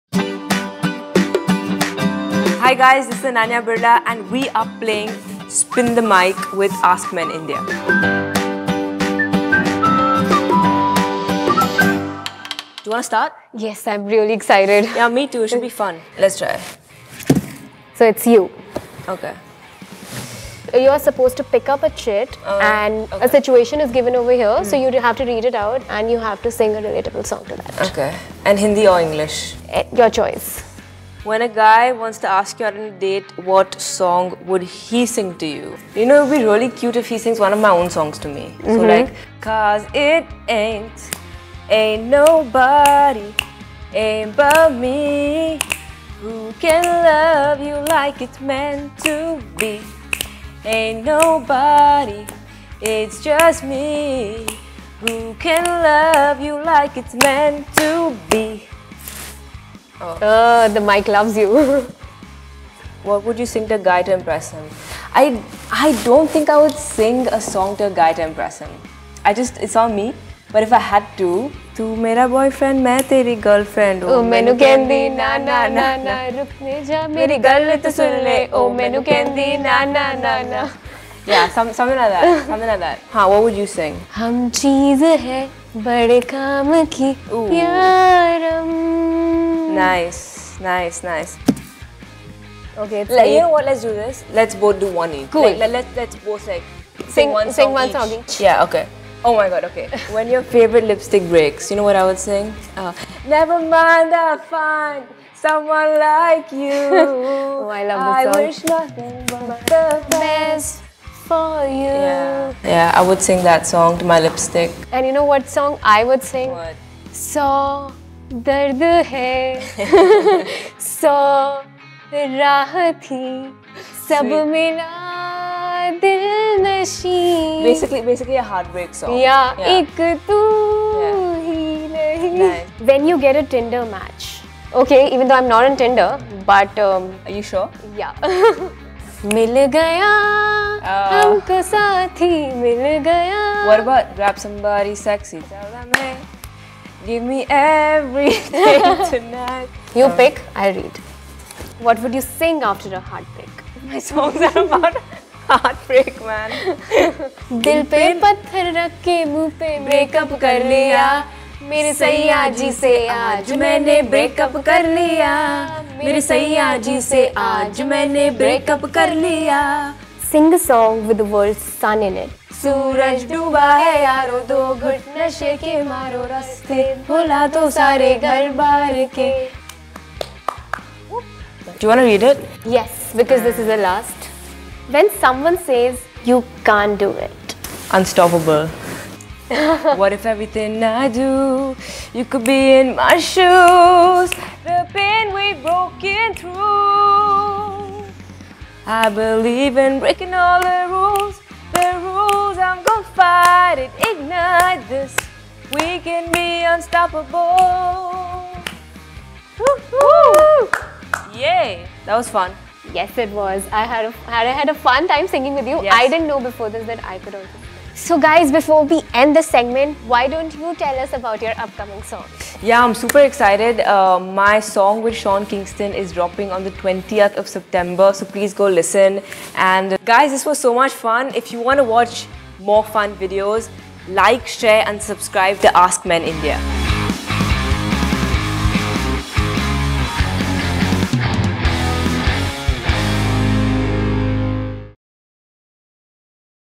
Spin The Mic Ft. Ananya Birla | AskMen India
In this week's 'Spin The Mic', we have the amazing Ananya Birla with us to play this game. Watch what happens when this musical sensation decides to have some fun with her tunes.